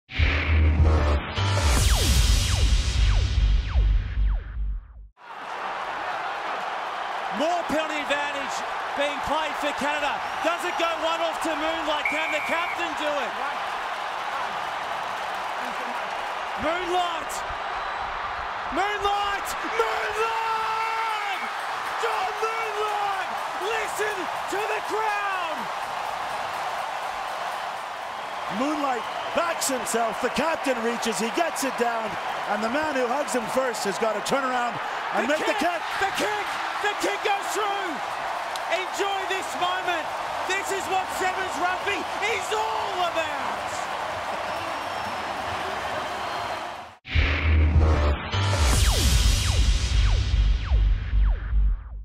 Canada win sevens THRILLER in Vancouver!
Canada win a thrilling Bowl final at the Canada Sevens

Follow World Rugby on social media:Official Website! to our YouTube Channel For More Great Videos us on Twitter us on Facebook us on Instagram